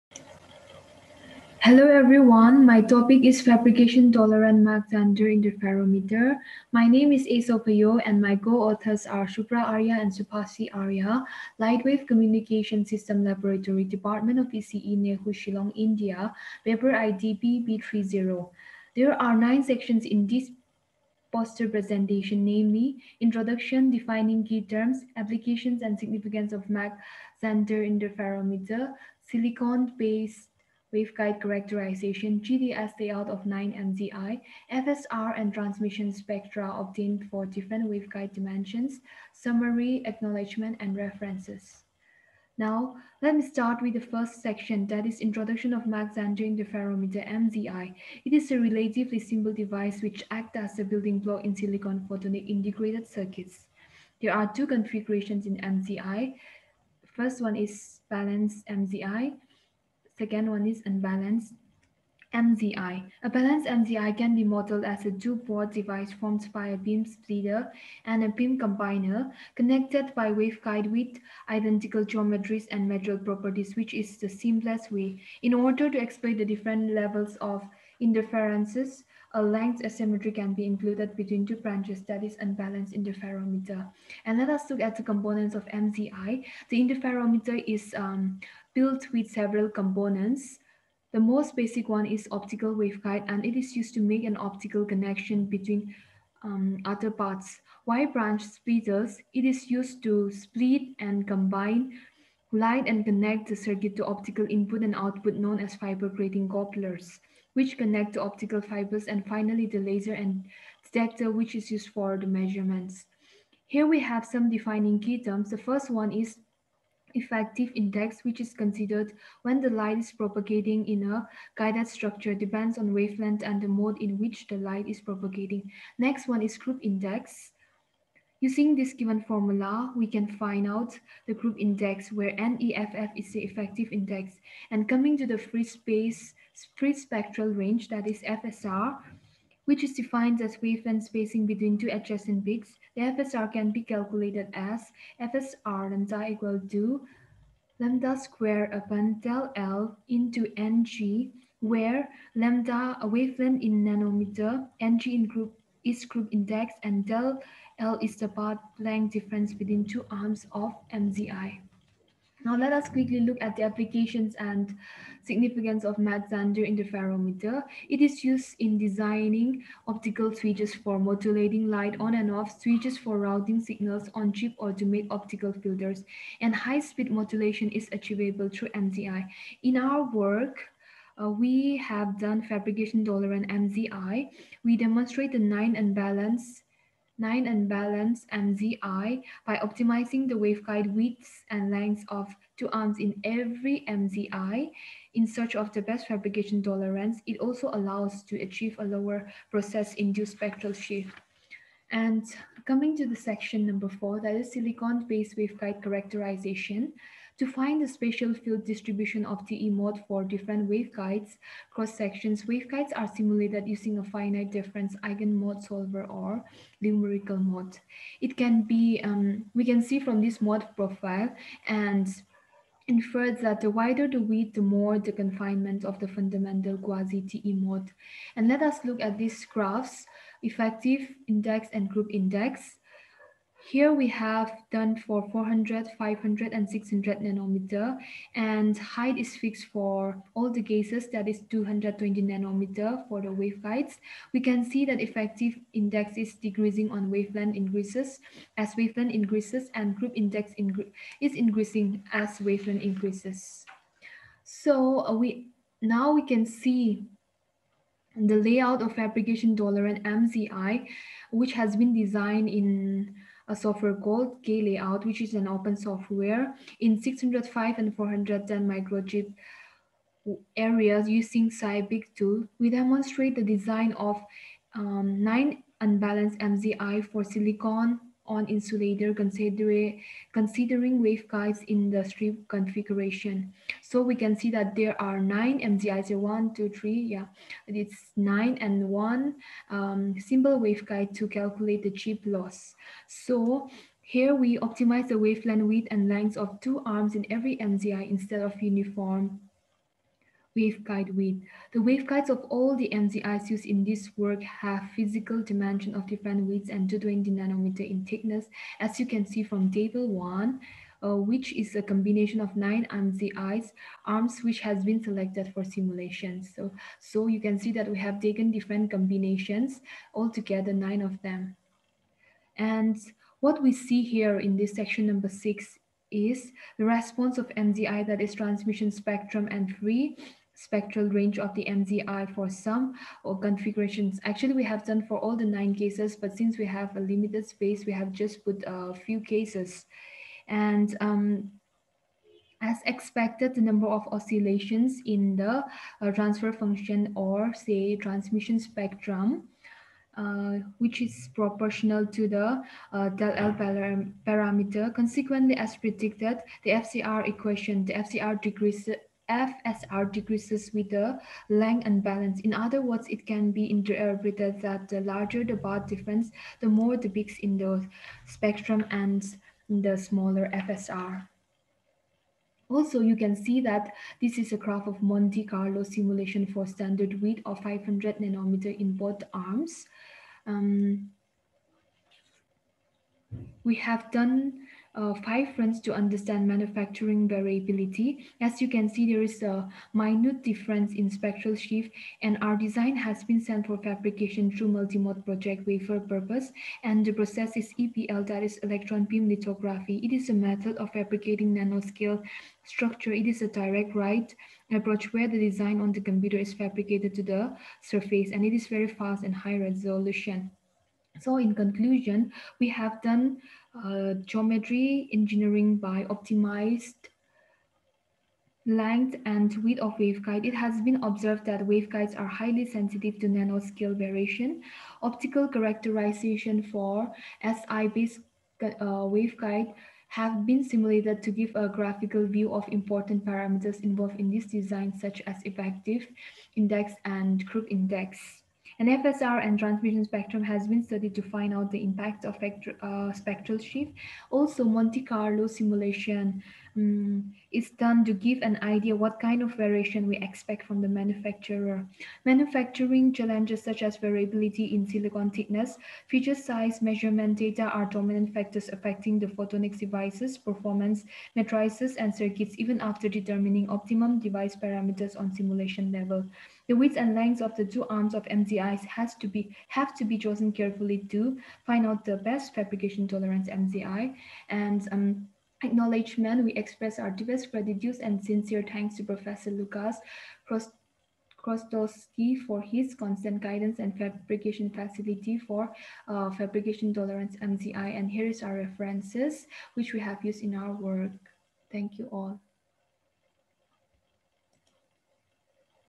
Fabrication Tolerant Mach–Zehnder Interferometer
The research on Fabrication Tolerant Mach–Zehnder Interferometer was presented by A. Sophayo of LWCS Laboratory, ECE Department NEHU, Shillong in INTERNATIONAL CONFERENCE ON RECENT TRENDS IN PHOTONICS (NPS-21) International School of Photonics, Cochin University of Science and Technology during 1st March 2021